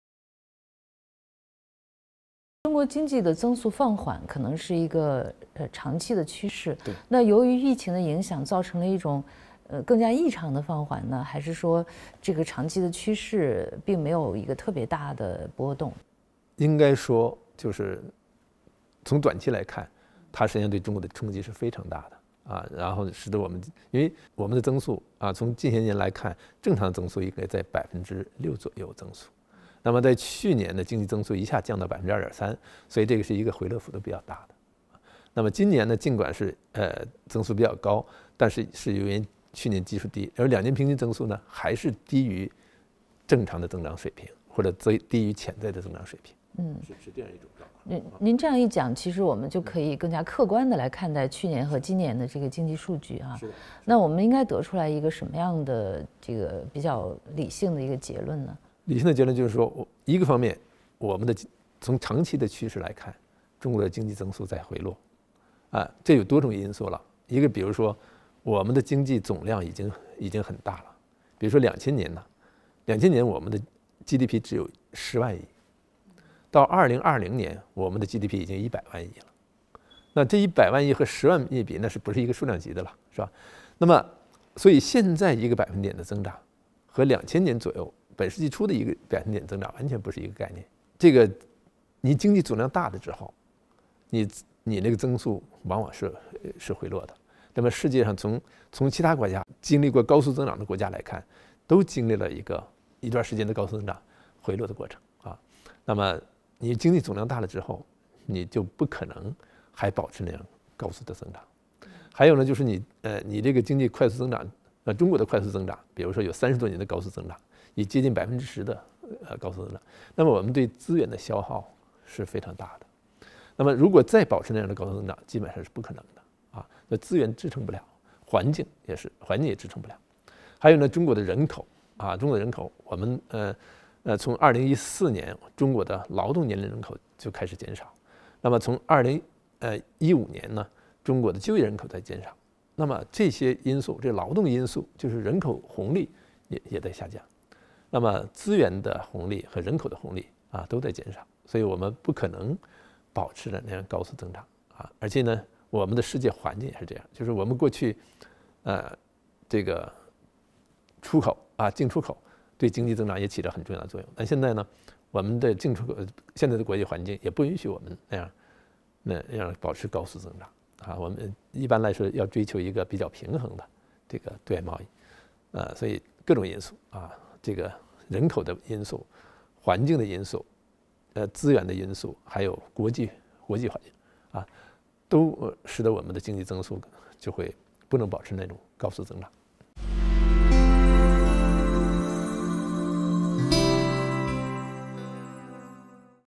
【清华大学】许宪春：诸多红利已经过去，中国经济增速放缓
人文清华短视频：人文清华短视频：国家统计局原副局长、清华大学经济管理学院教授许宪春表示中国的经济增速由高速增长逐渐放缓，在人口、资源与环境等因素的限制下，经济增速放缓将成为长期趋势。